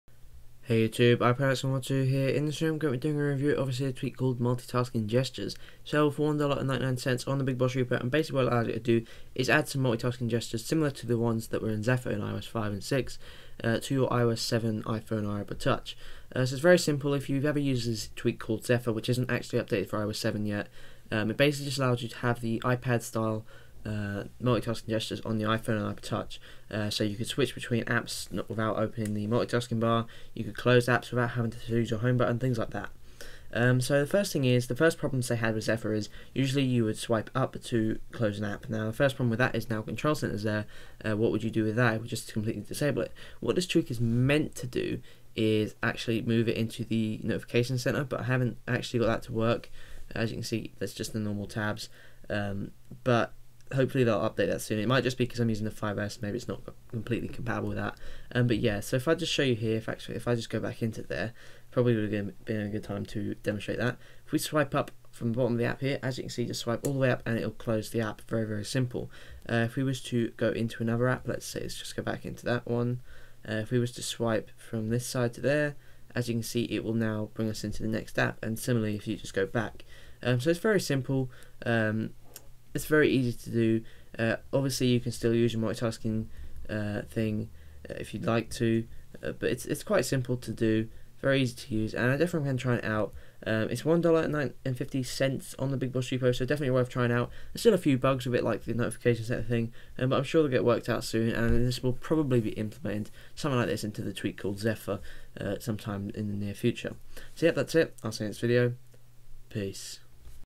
Multitasking Gestures for the iPhone/iPod Touch With iOS 7 | MultitaskingGestures Cydia Tweak Review
Name: MultitaskingGestures
Price: $1.50
Repo: BigBoss
Developer: Hamza Sood

Leave a comment or PM me if you have any questions.